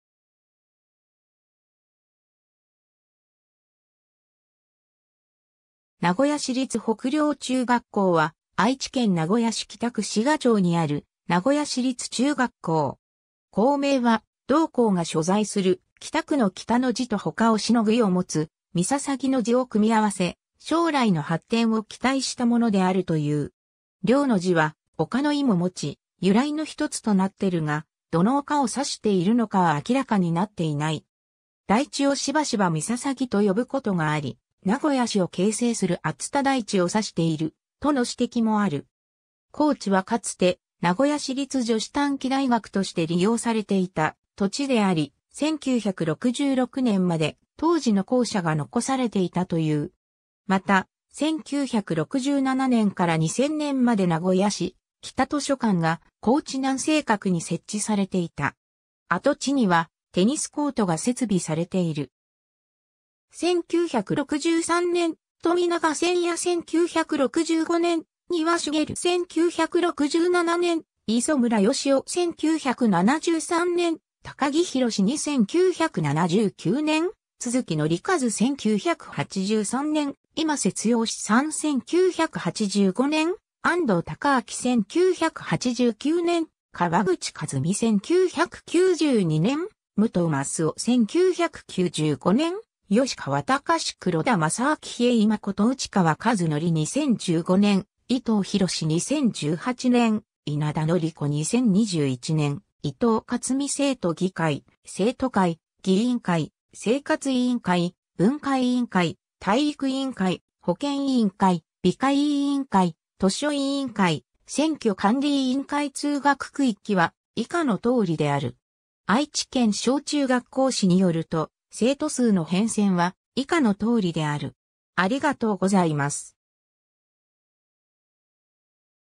名古屋市立北陵中学校
名古屋市立北陵中学校（なごやしりつ ほくりょうちゅうがっこう）は、愛知県名古屋市北区志賀町にある名古屋市立中学校。
 校名は同校が所在する北区の北の字と他を凌ぐ意を持つ陵の字を組み合わせ、将来の発展を期待したものであるという。
陵の字は丘の意も持ち、由来のひとつとなってるが、どの丘を指しているのかは明らかになっていない。
台地をしばしば陵と呼ぶことがあり、名古屋市を形成する熱田台地を指している、との指摘もある。
校地はかつて名古屋市立女子短期大学として利用されていた土地であり、1966年（昭和41年）まで当時の校舎が残されていたという。
また、1967年（昭和42年）から2000年（平成12年）まで名古屋市北図書館が校地南西角に設置されていた。
跡地にはテニスコートが設備されている。
 1963年（昭和38年） - 冨永 仙弥 1965年（昭和40年） - 丹羽 茂 1967年（昭和42年） - 磯村 喜雄 1973年（昭和48年） - 高木 弘二 1979年（昭和53年） - 都築 則一 1983年（昭和58年） - 今瀬 剛三 1985年（昭和60年） - 安藤 隆昭 1989年（平成元年） - 川口 和美 1992年（平成4年） - 武藤 益夫 1995年（平成7年） - 吉川 隆 黒田 正明 日江井 真 内川 和則 2015年（平成27年） - 伊藤 博 2018年（平成30年） - 稲田典子 2021年　(令和3年)　- 伊藤克実 生徒議会、生徒会、議員会、生活委員会、文化委員会、体育委員会、保健委員会、美化委員会、図書委員会、選挙管理委員会 通学区域は以下の通りである。
 『愛知県小中学校誌』（2018年）によると、生徒数の変遷は以下の通りである。